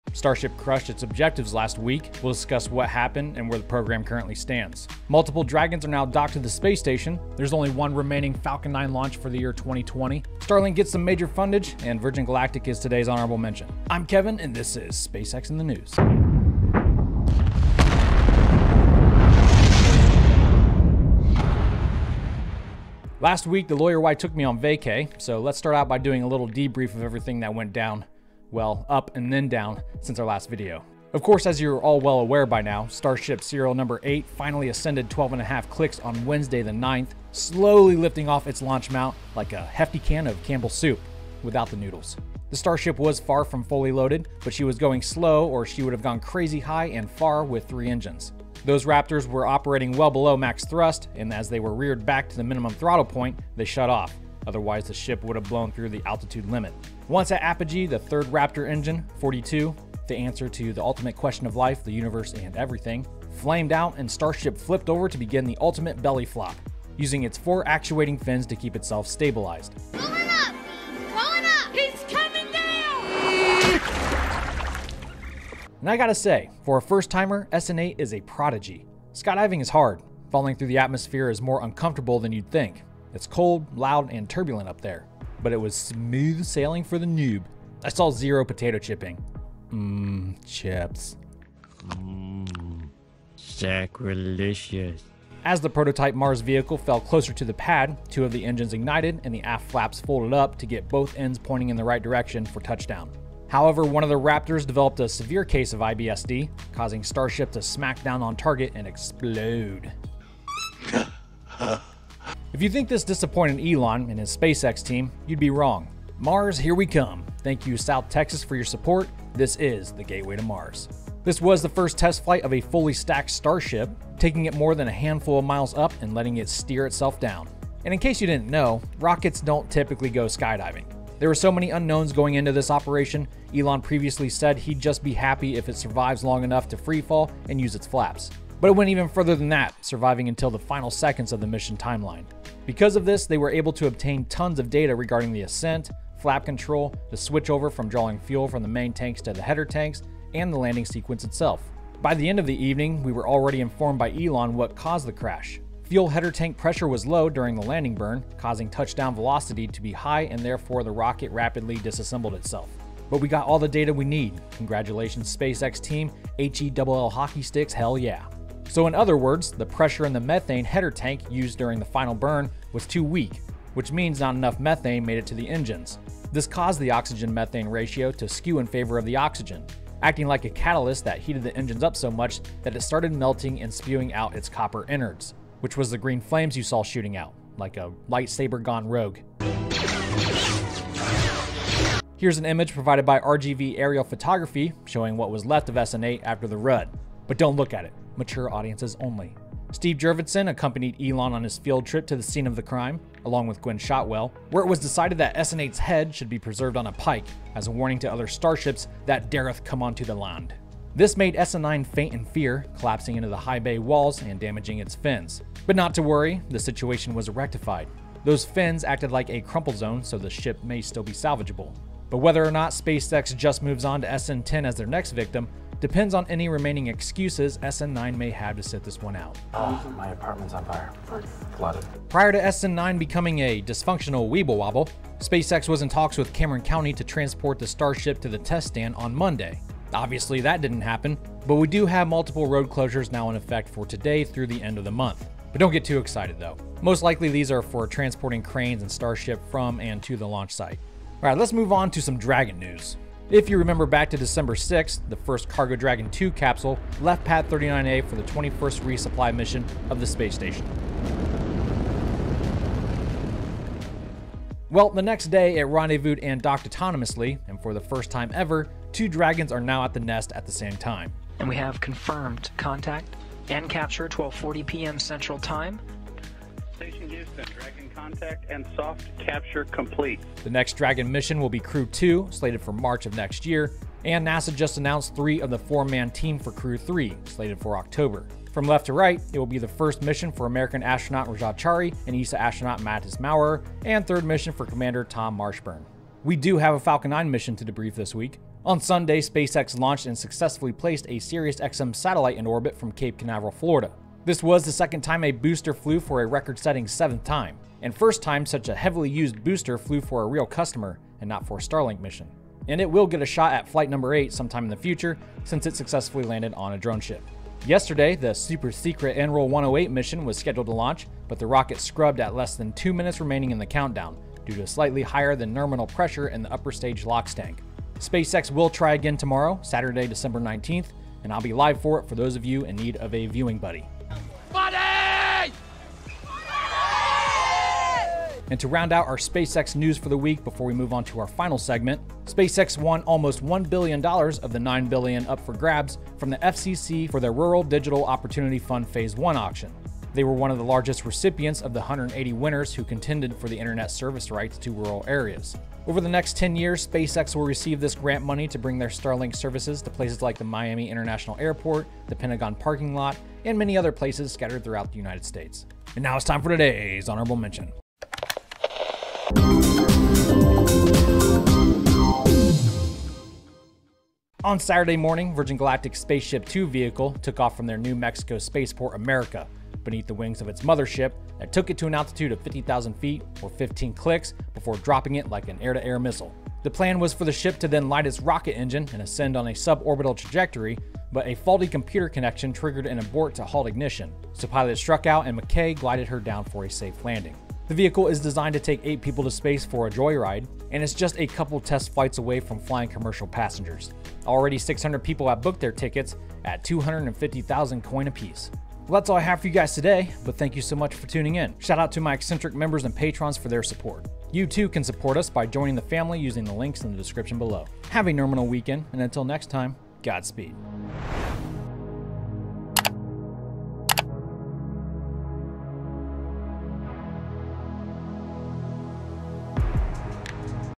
Starship's Current Status | SpaceX in the News
SpaceX Starship's Current Status | SpaceX in the News Episode 190. Today we debrief SN8's 12.5 km flight, and catch up on Starship's current status. Look at what's going on with Dragon. Recap a recent Falcon launch. Briefly talk Starlink, and finish with today's Honorable Mention.

XCENTRIC PATCHES

THUMBNAIL IMAGE: SpaceX